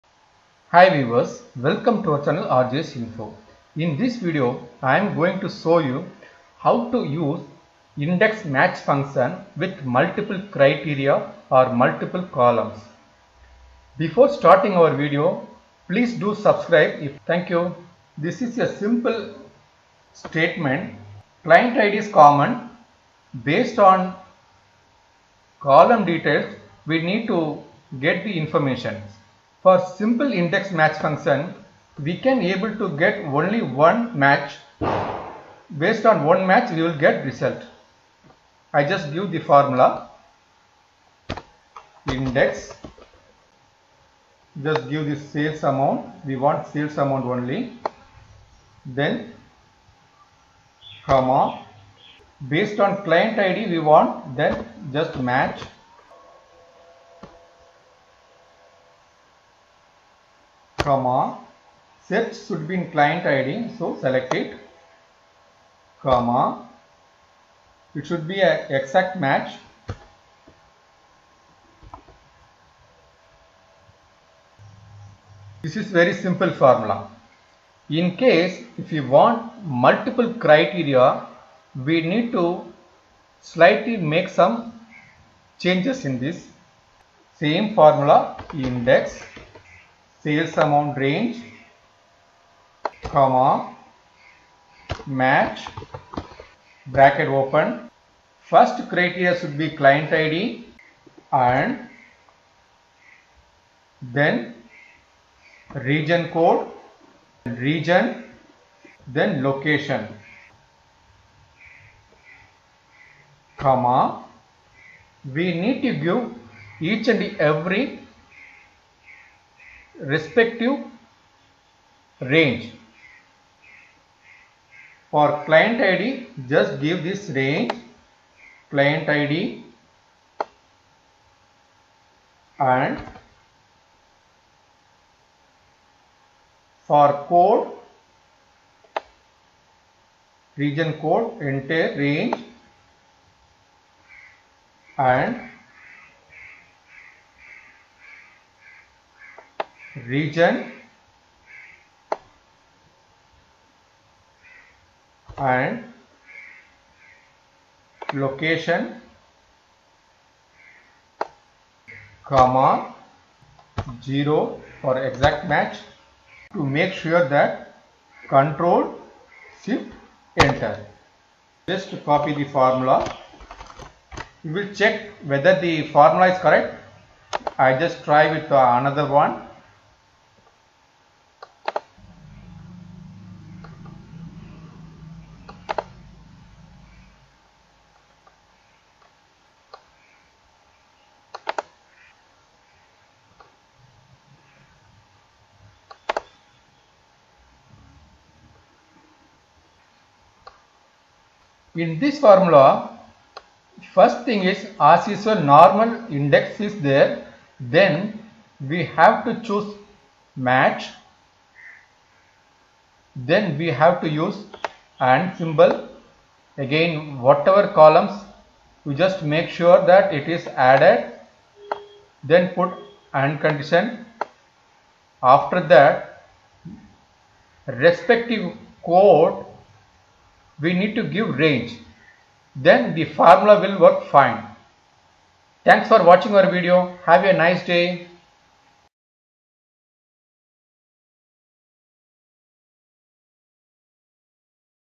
Index match with multiple criteria | excel multiple columns match
Discover how to use the INDEX MATCH function in Excel to perform multiple criteria matches in this tutorial. Learn step-by-step how to search for data across multiple columns and retrieve the corresponding values using this powerful formula combination. Whether you're a beginner or an experienced user, this video will provide clear examples and explanations to enhance your Excel skills. Follow along and master the art of multi-criteria matching today!
Excel sheets related to Basic to Advanced Excel Skills are available for practice,